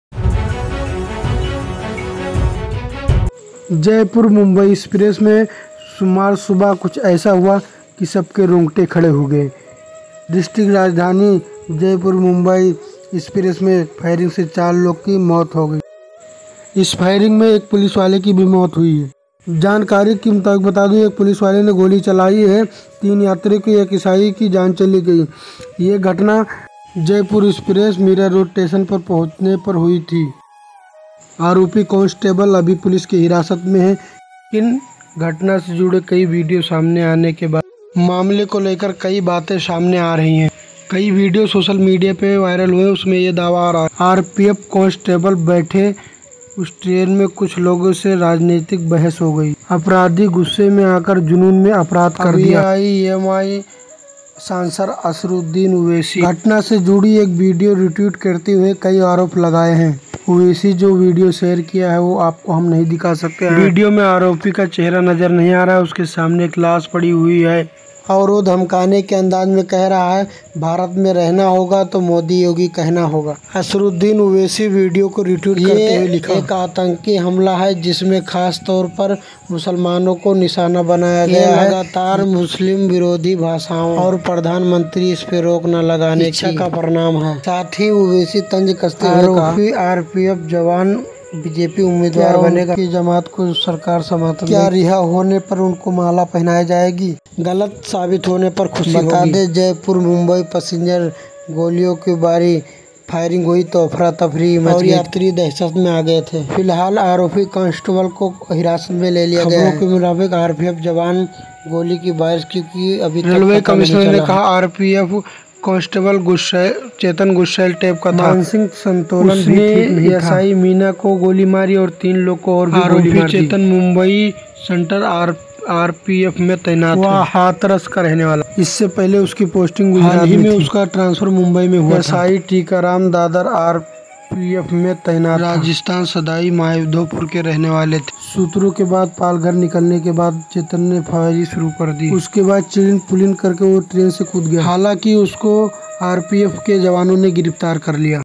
Jaipur Mumbai Train Firing: Owaisi बोले- ये कांड मुस्लिमों पर आतंकी हमला, PM Modi को भी लपेटा
Jaipur Mumbai Train Firing: Owaisi बोले- ये कांड मुस्लिमों पर आतंकी हमला, PM Modi को भी लपेटा@shajadsalmani5482